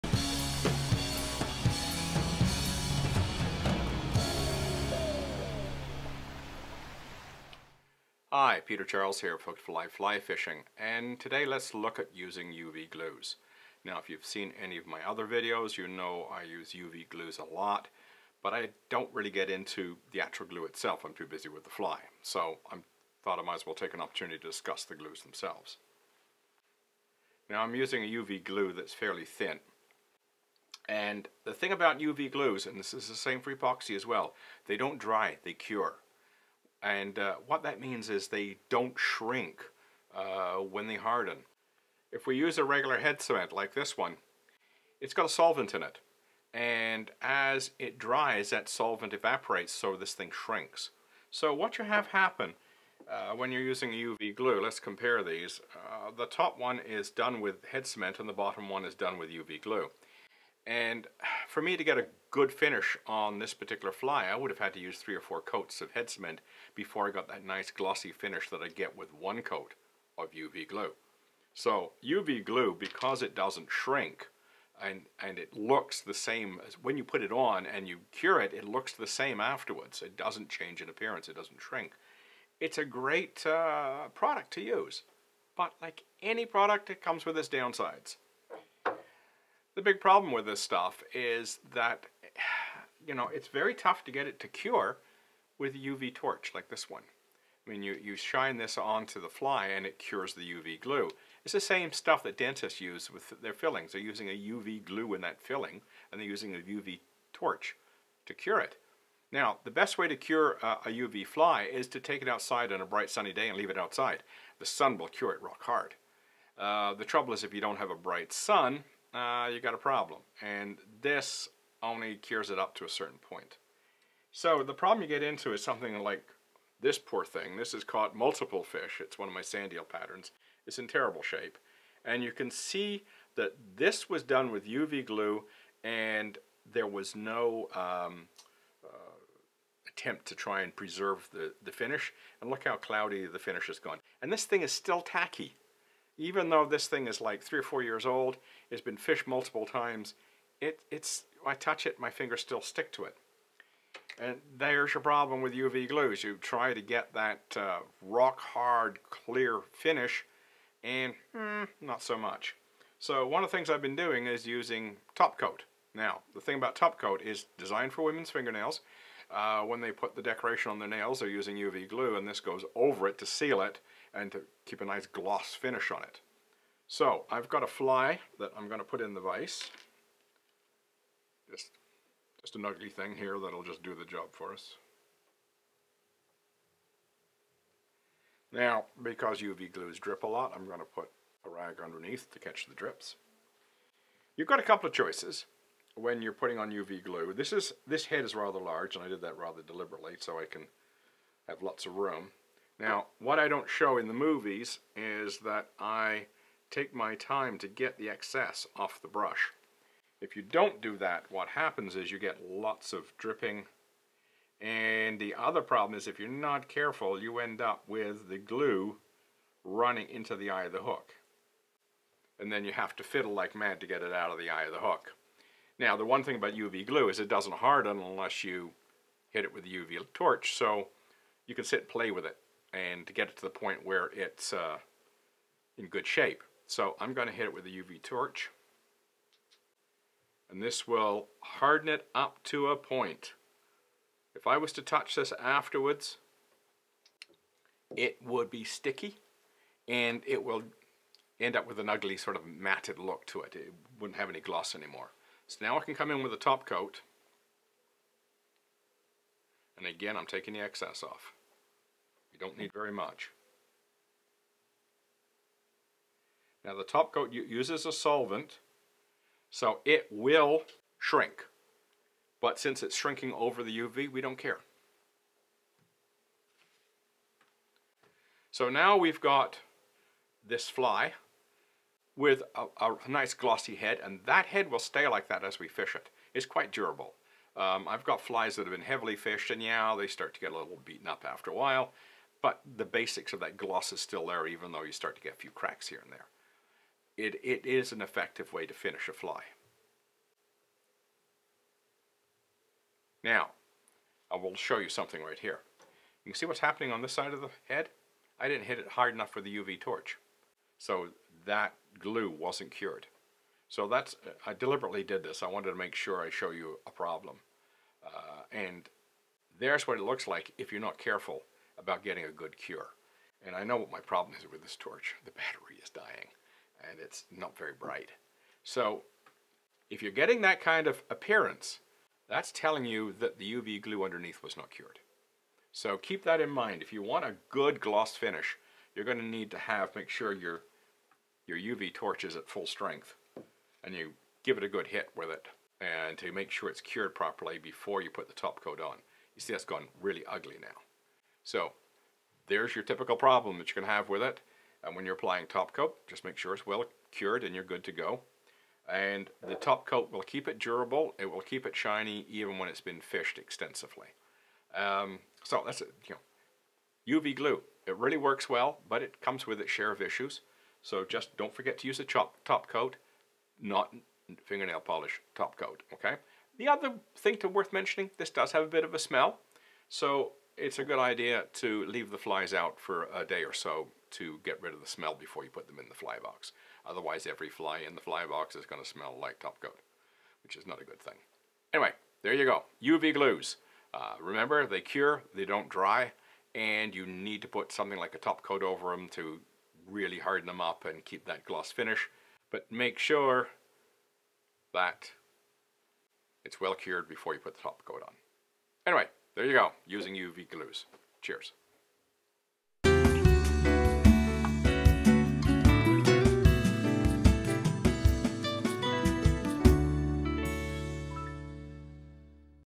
Beginner's Fly Tying Series: Tricks & Troubles - Using UV Glues
UV Glues are a great product for getting a very nice looking head quickly, but they do have some issues. Here's how to get the best results out of a UV glue.